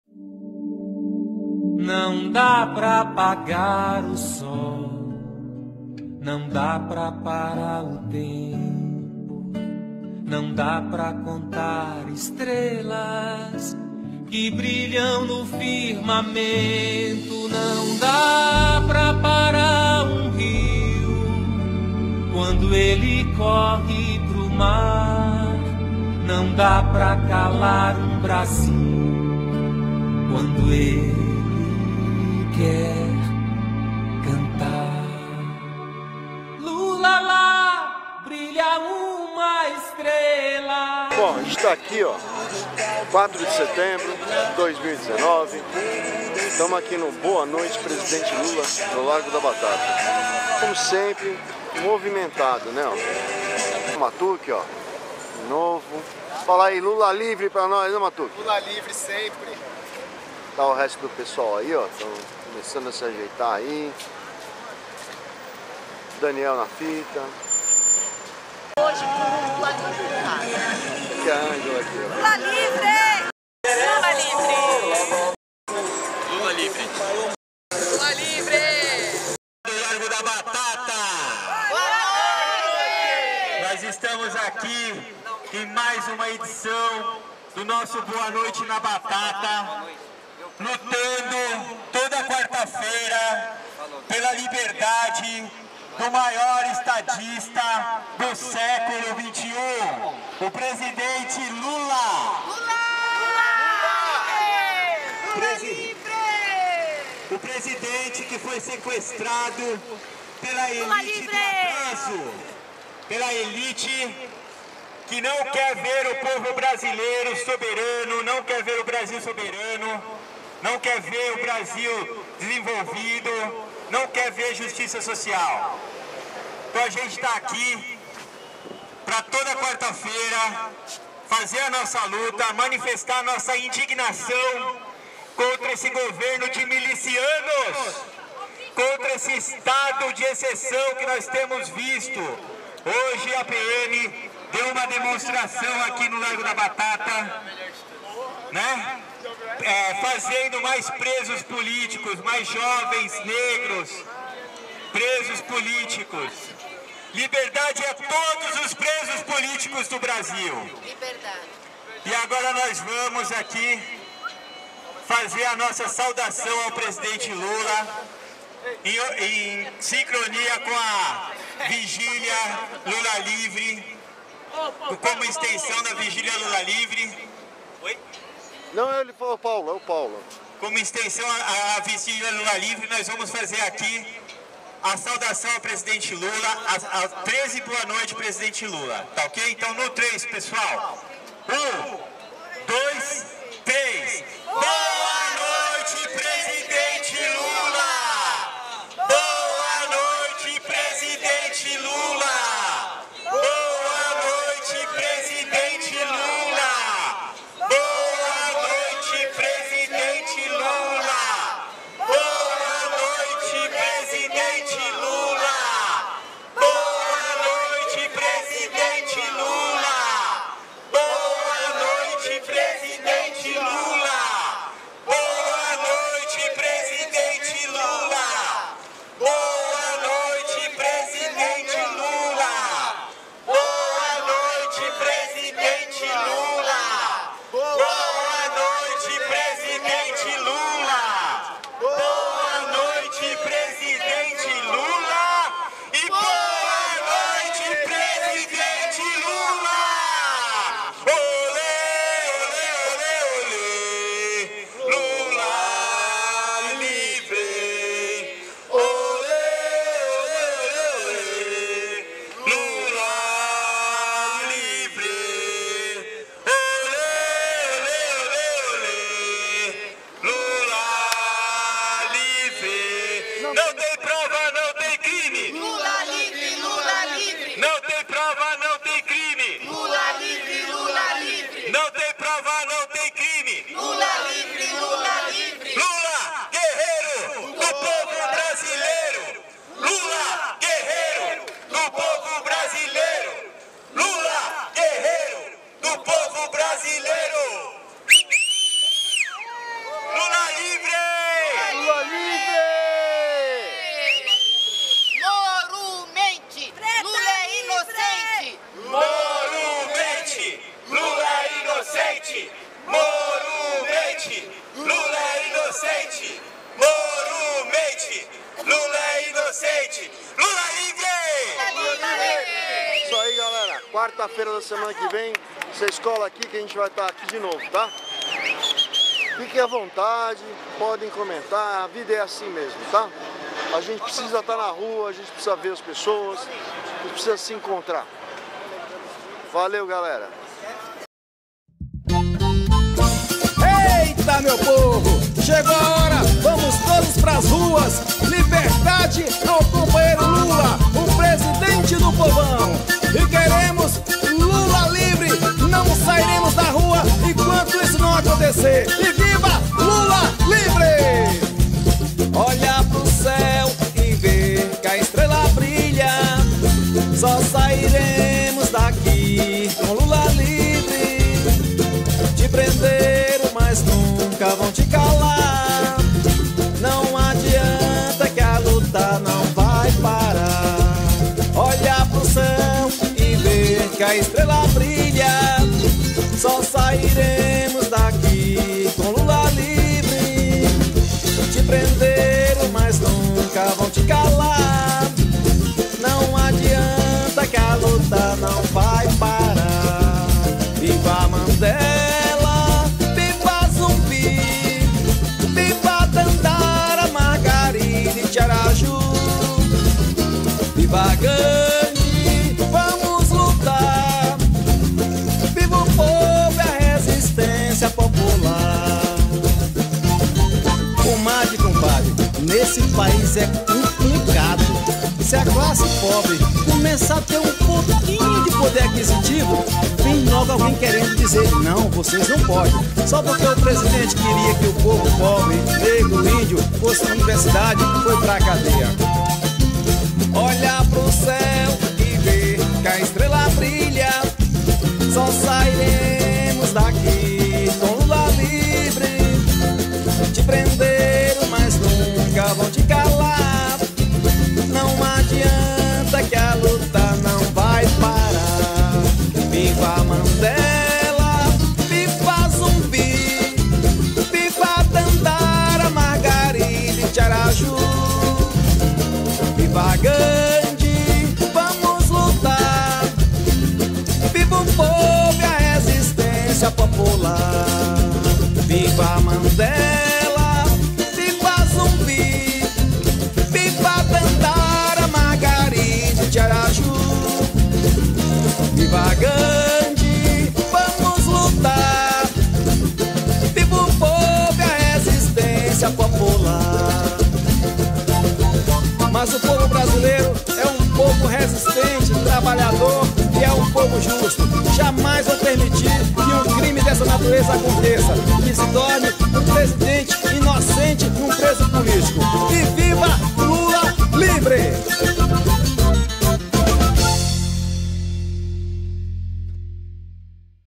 Boa noite presidente Lula 04.09.2019
Ato do CCD-LL por Lula Livre

No largo da Batata o coletivo CorAção do Comite Lula Livre Zona Oeste com apoio de outros coletivos da cidade realiza o ato em homenagem ao nosso presidente que no momento é um preso político desde o dia 07/04/2018.